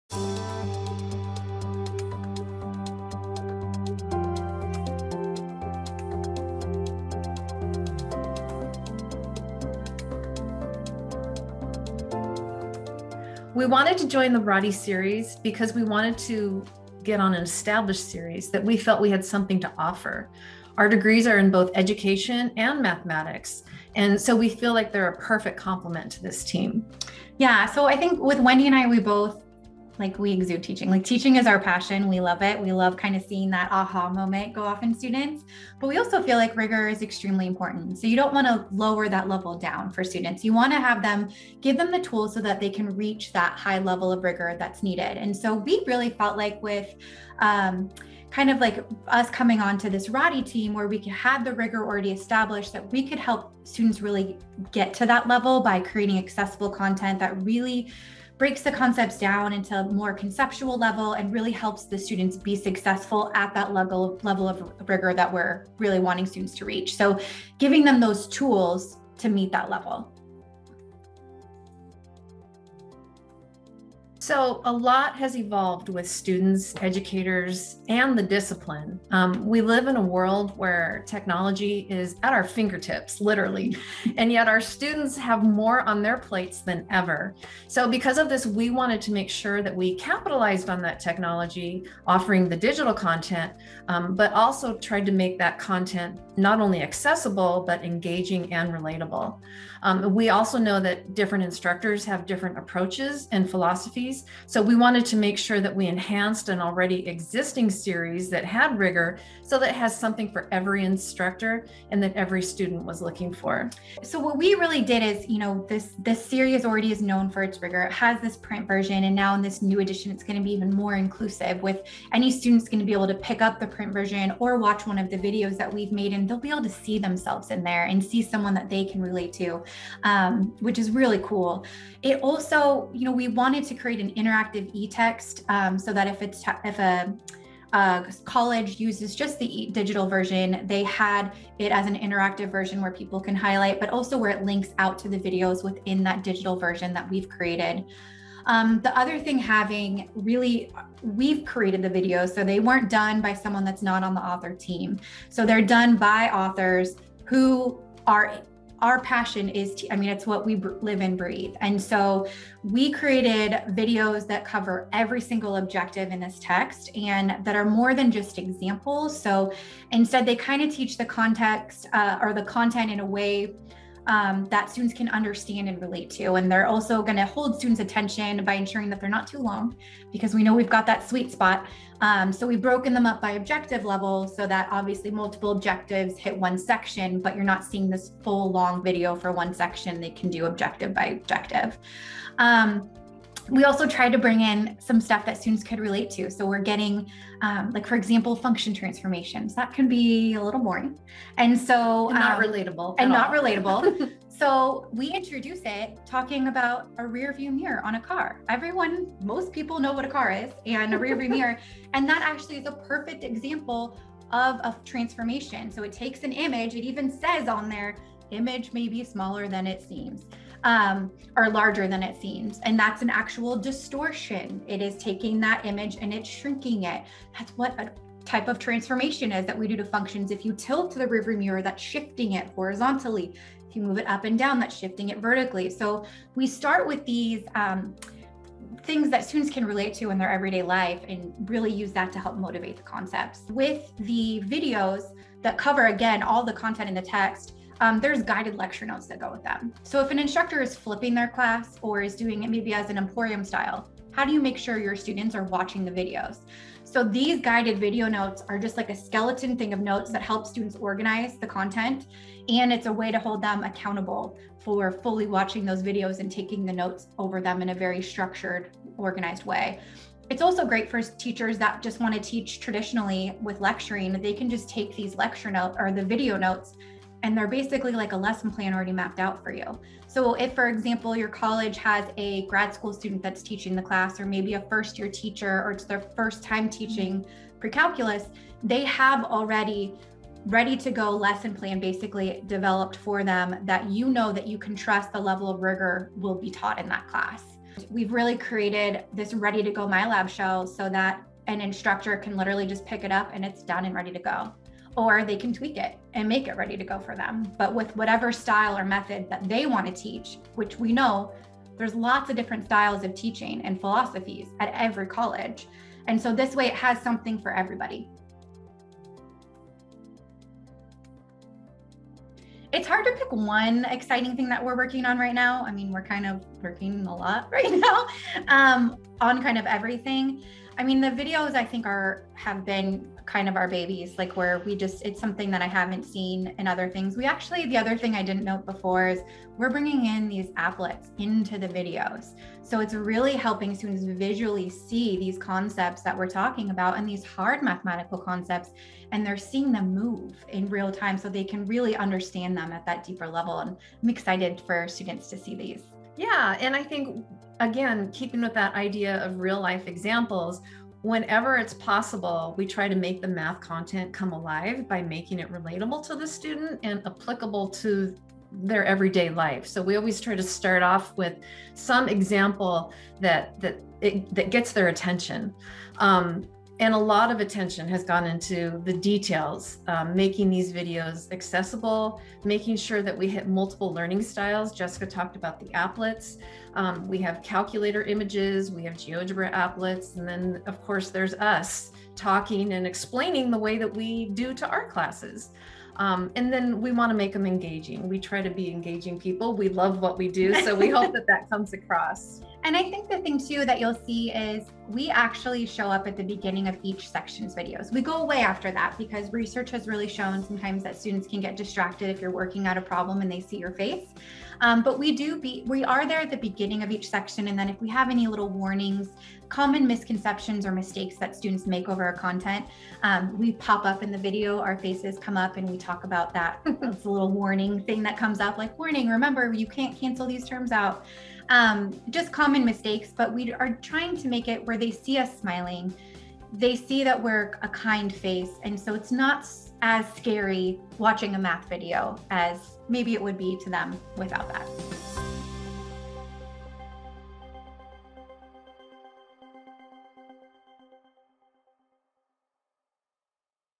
Jessica Bernards and Wendy Fresh join Ratti, McWaters & Skrzypek Precalculus Series
Hear from new co-authors Jessica Bernards and Wendy Fresh on the Ratti, McWaters & Skrzypek Precalculus Series.